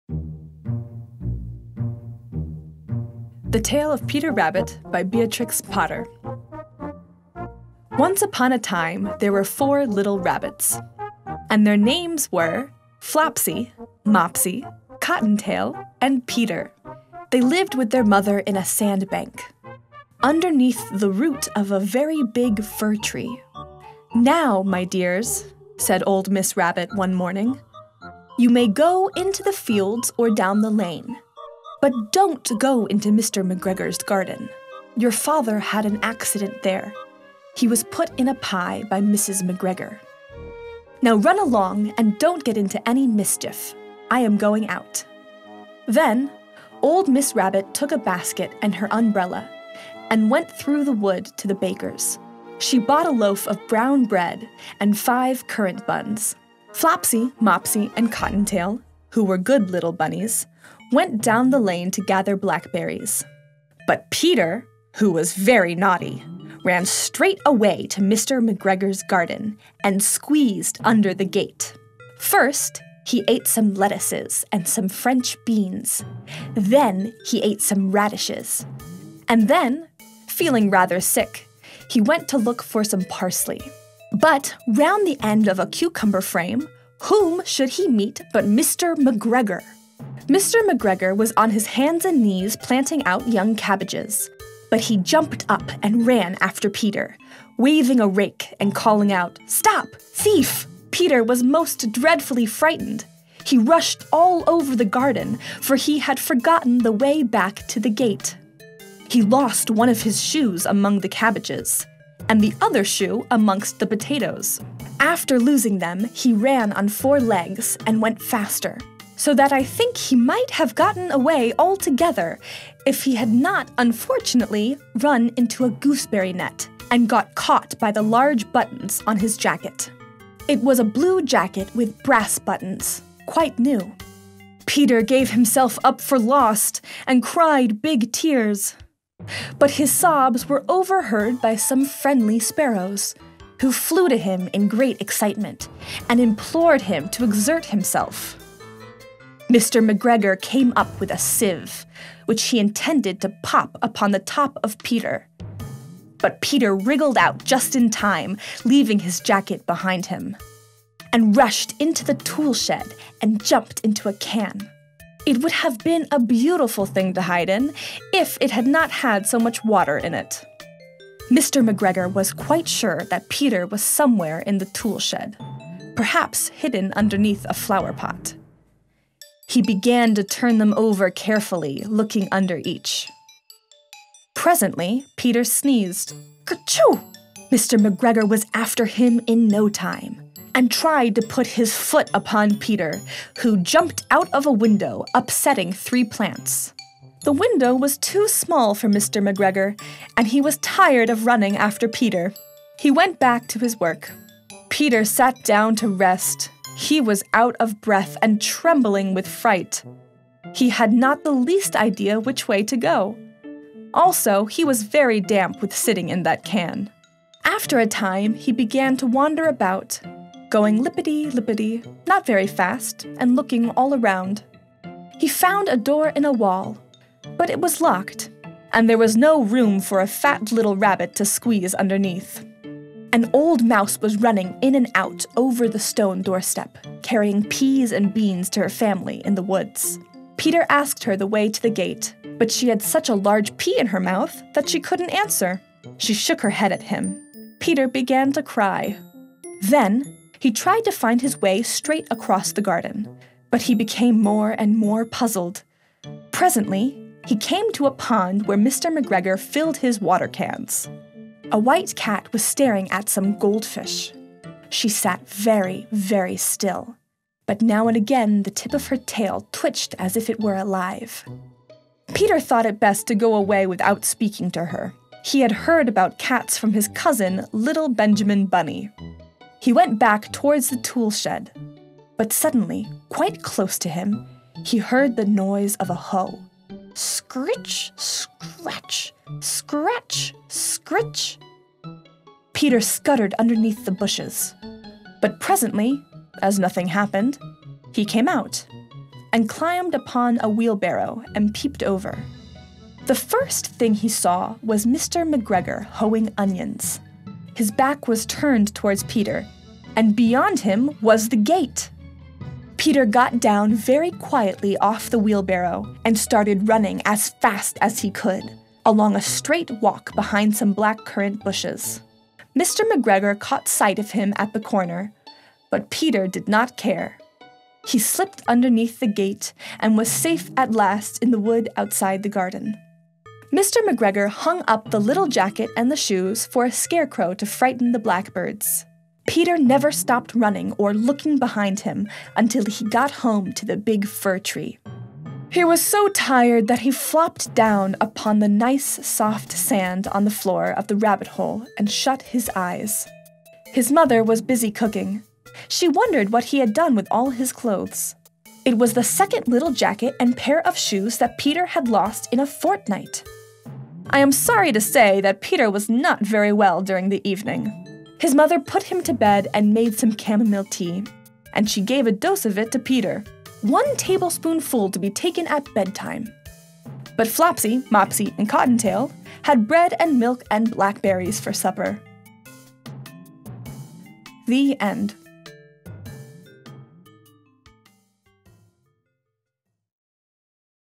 The Tale of Peter Rabbit (B. Potter)
Original story, written by Beatrix Potter. Animation with subtitles. Recorded by professional actress (Aran Savory).